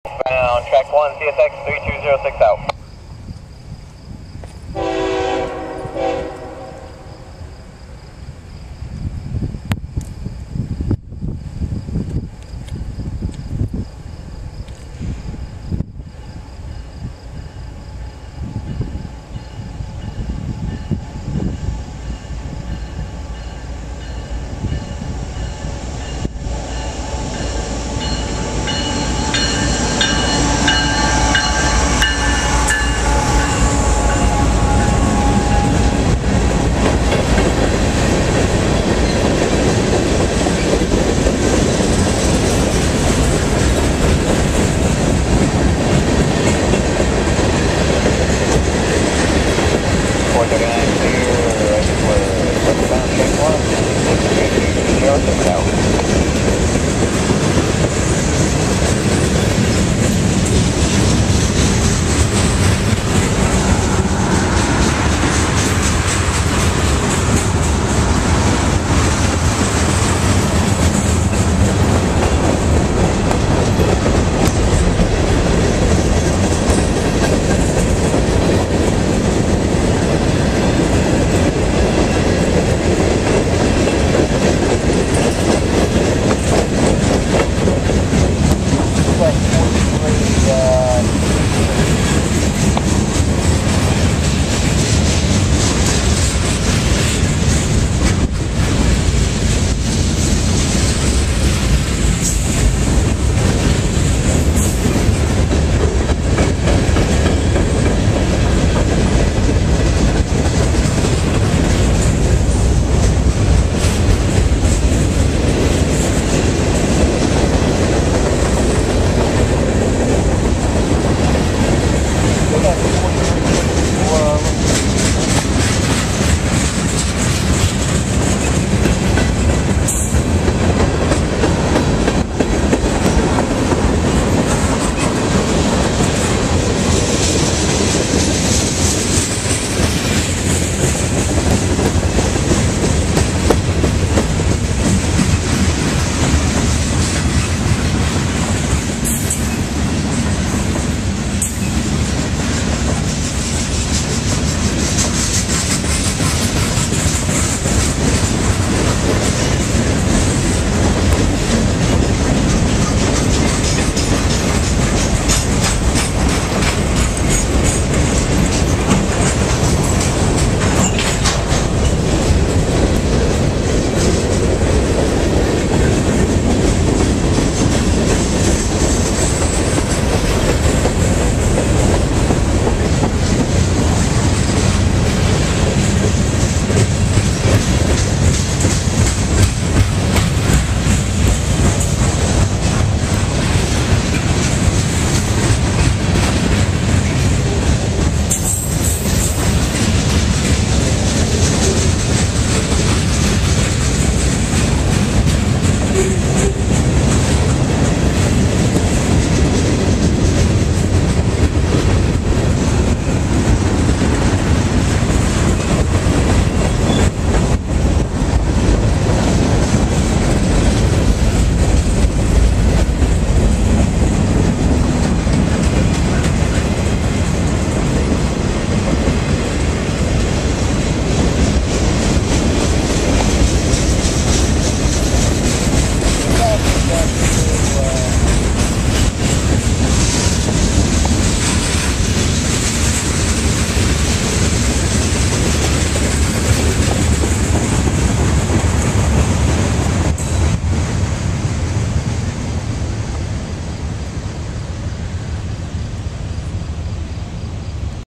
CSX Q439 HEADS WEST THRU YARDLEY PA, MP30.64 TREL
5/30/15 Here is CSX Q439 as it heads west through Yardley PA.
Power: CSX 3206 ES44AH and 7497 C40-8